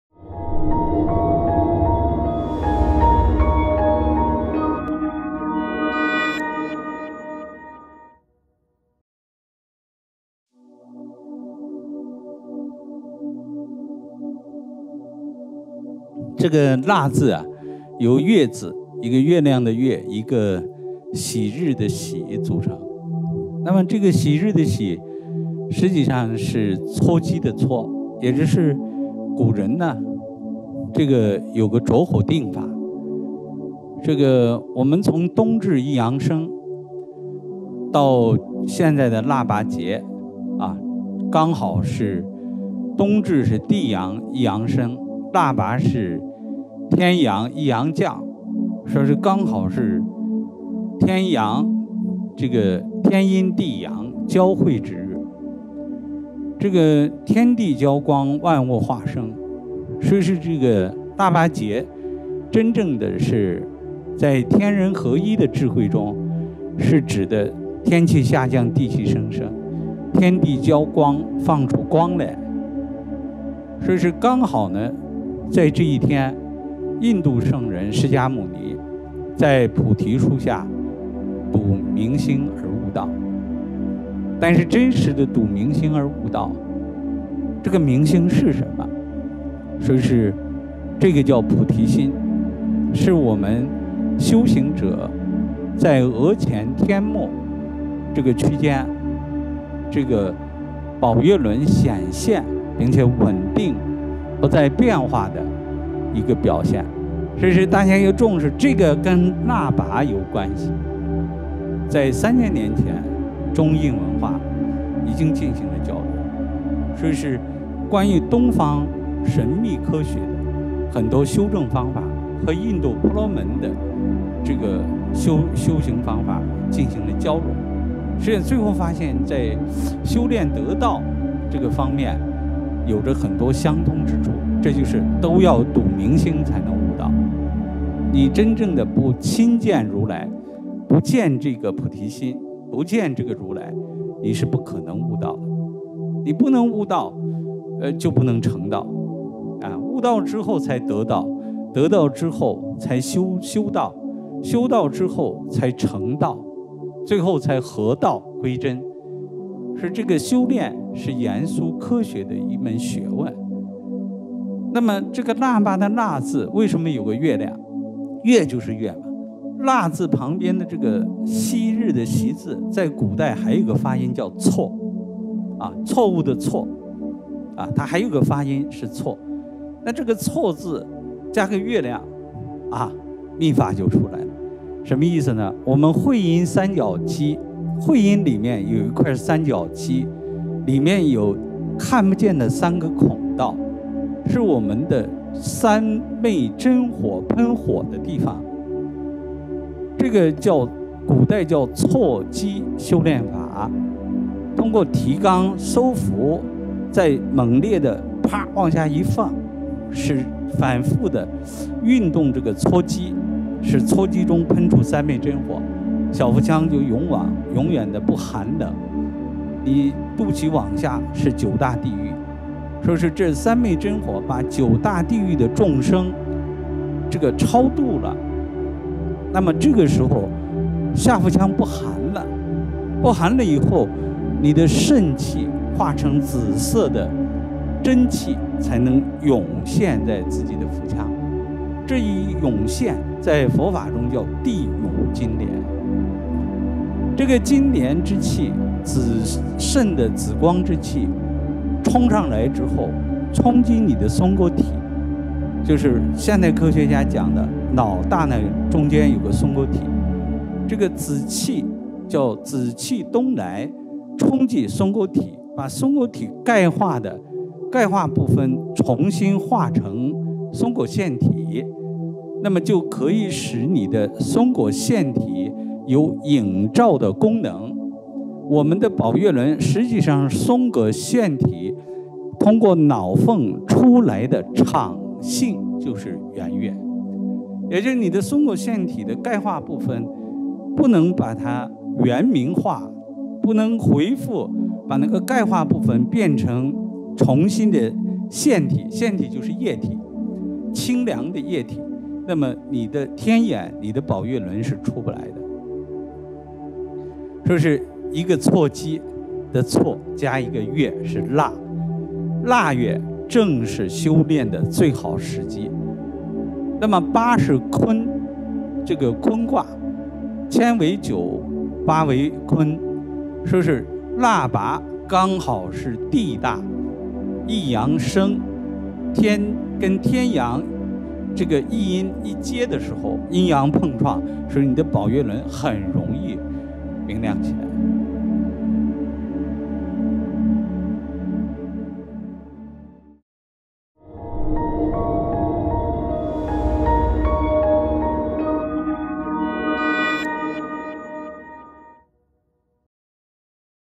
2021 纪念佛陀悟道日海陆空全球普度祈福消灾法会回顾短片 张卜生教授
张卜生教授

Europe 欧洲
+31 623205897